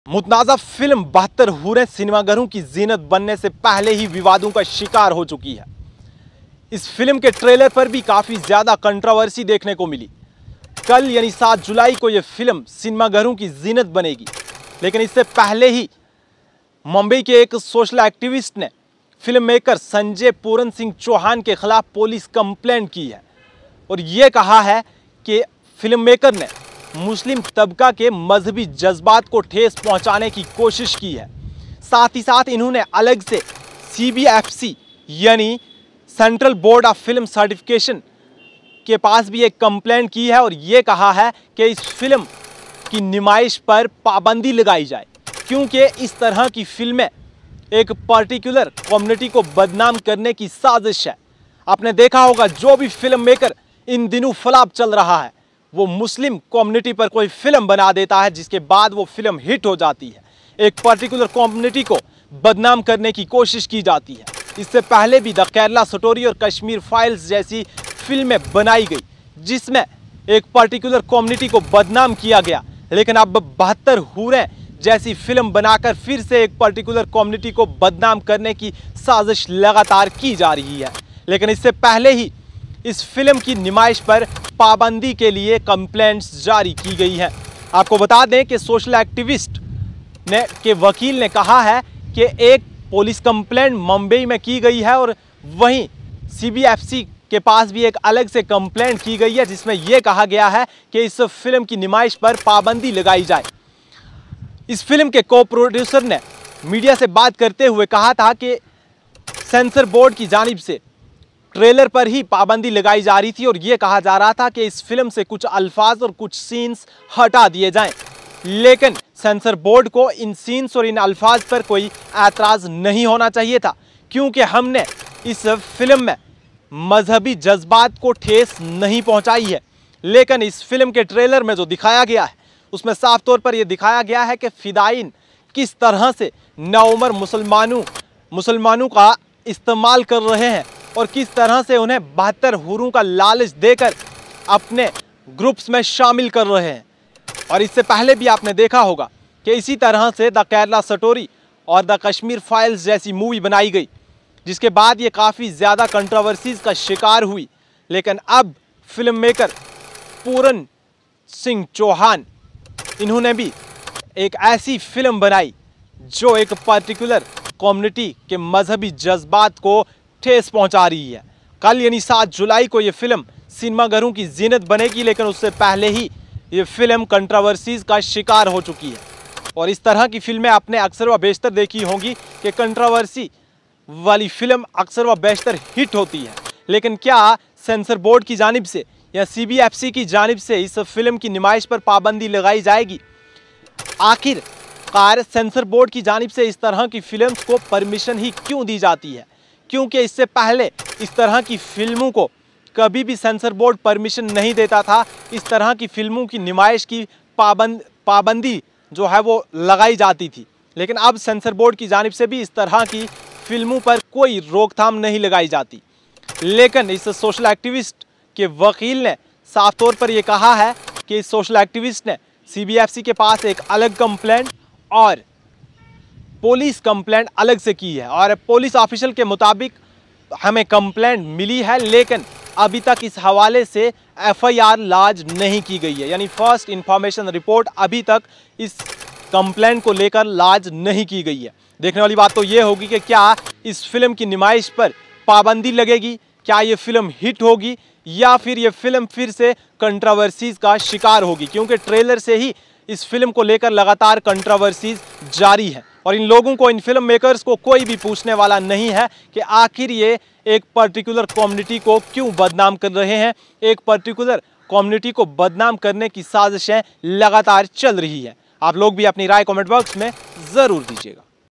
72 Hoorain’: Police complaint filed against filmmakers for hurting religious sentiments
72 Hoorain’: Police complaint filed against filmmakers for hurting religious sentiments